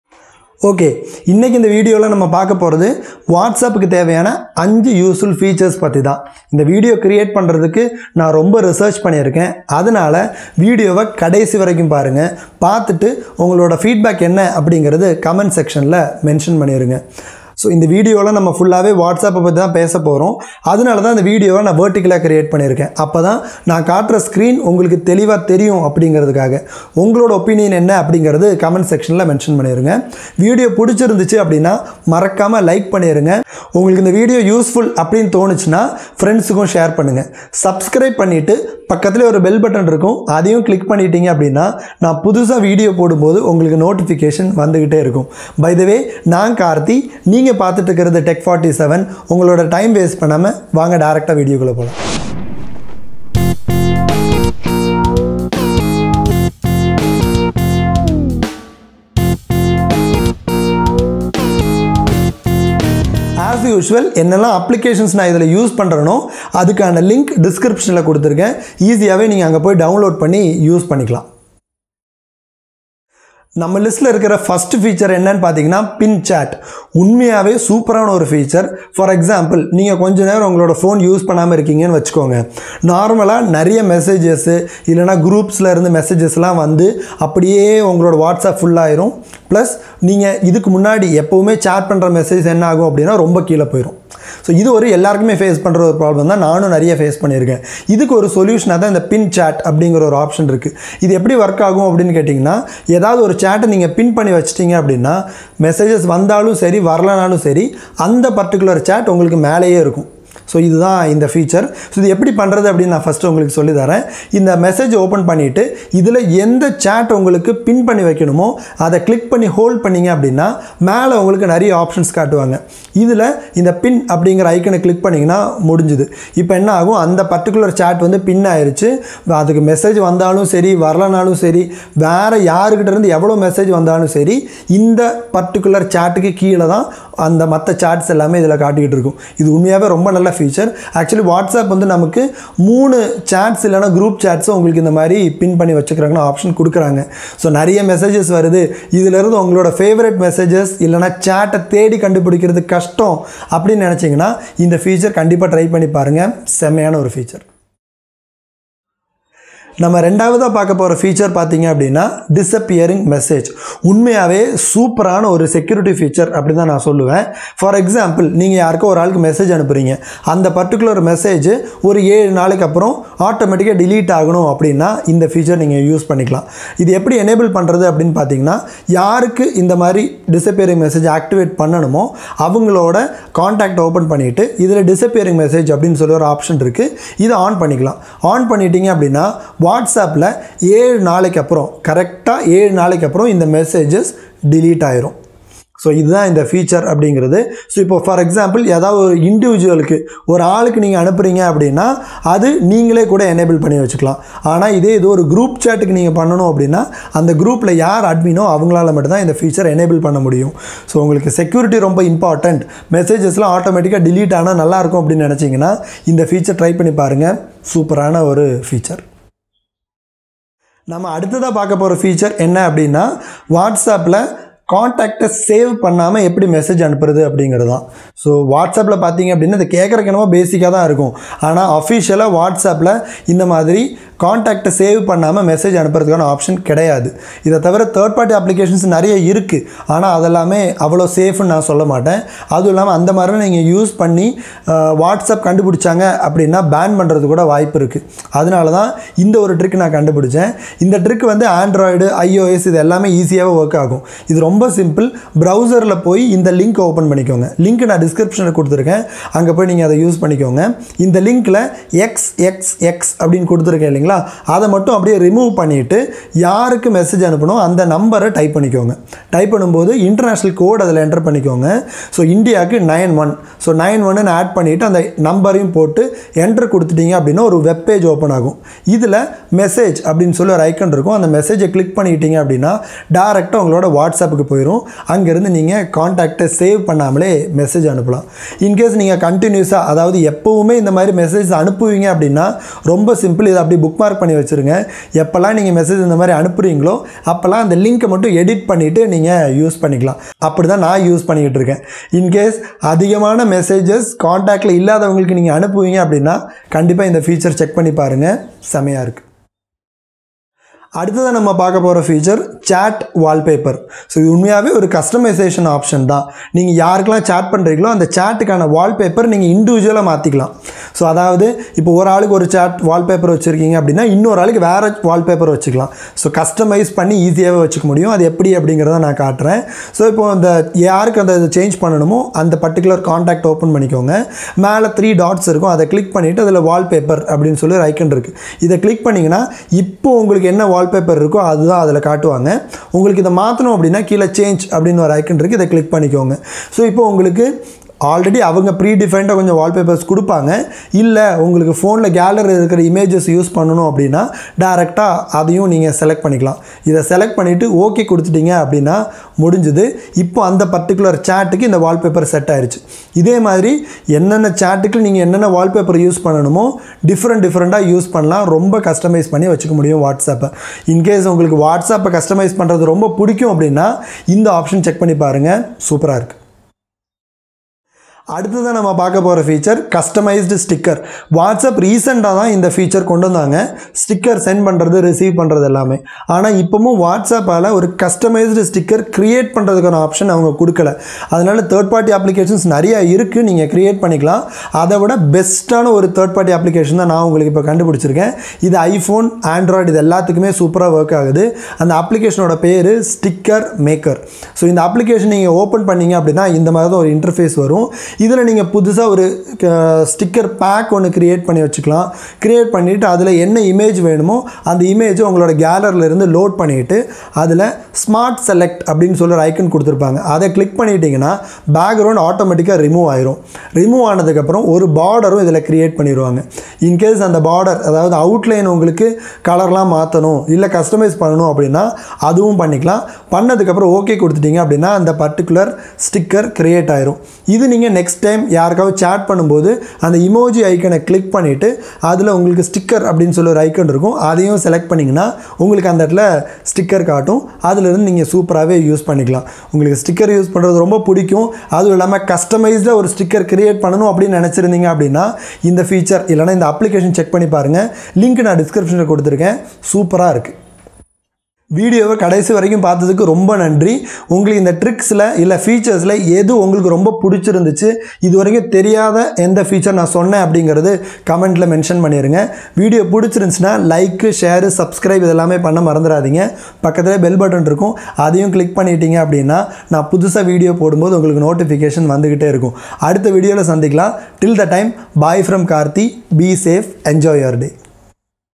Whatsapp Tips and Tricks in Tamil - Mar’21
In this video I will show you 5 useful features which you can use on whatsapp to make it more productive.

This list is for the month of Feb’2021, I have done a good amount of research in getting this list created.

So please watch the video till the end and don’t forget to share your valuable feedback via the comment section below.

Since we are only gonna discuss whatsapp, I have created this video as a vertical one to make the screen more visible to you all.

What's your opinion on this??? Comment down !!!